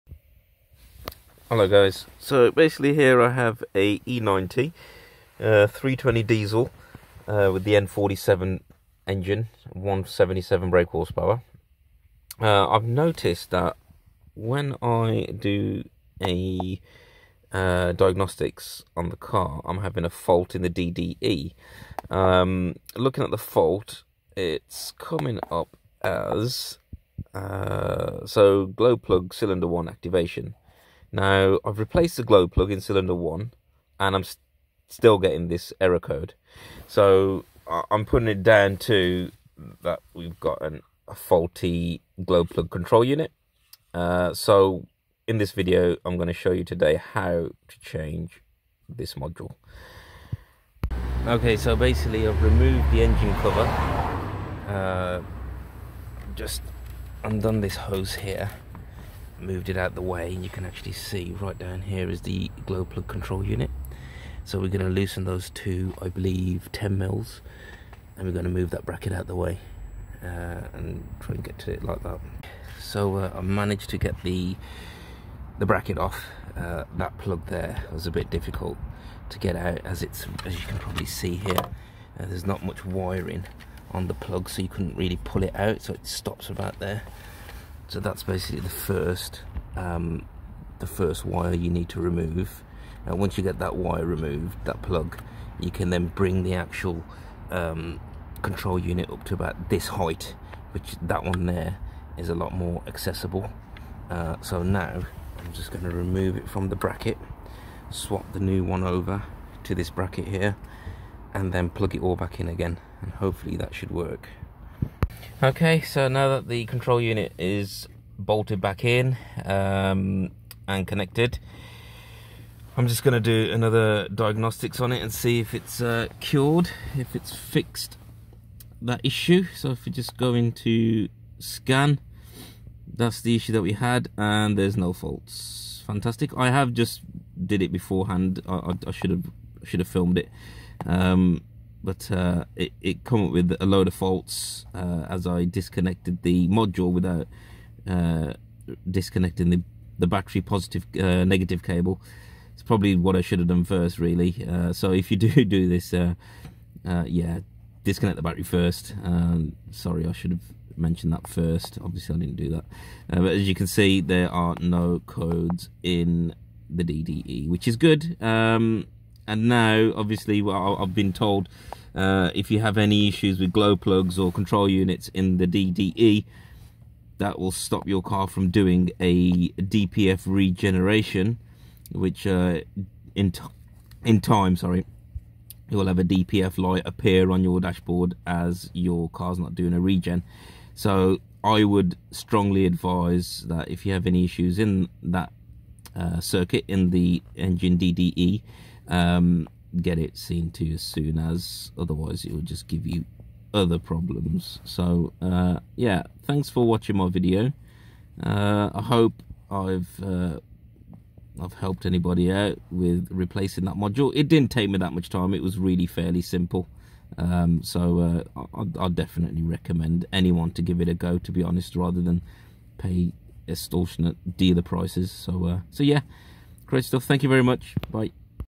BMW glow plug control unit replace N47 e90
How to replace a Glow Plug Control Unit on a bmw N47 engine